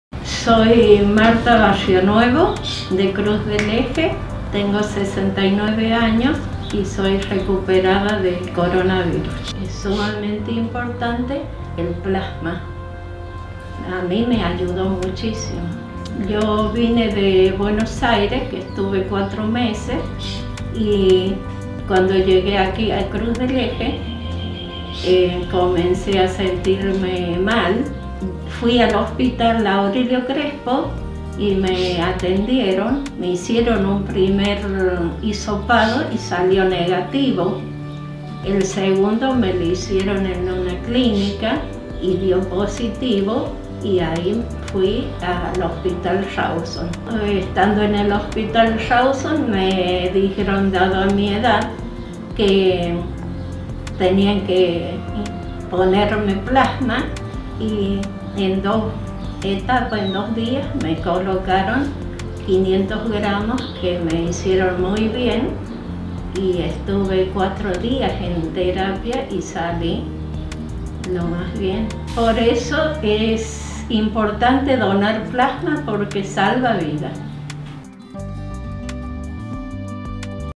¡Si tuviste coronavirus tu plasma puede ayudar a otros a superarlo!
👆Conocé la historia de Marta Barrionuevo, que con 69 años se recuperó de coronavirus y recibió una transfusión de plasma en su tratamiento.